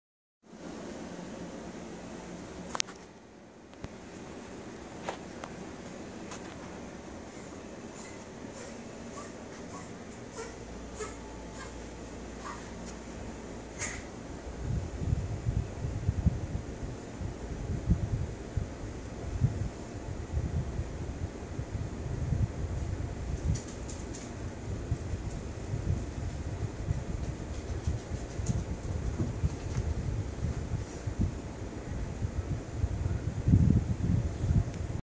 Chase grooming the new kitten
Chase took really fast to the newest addition to our family.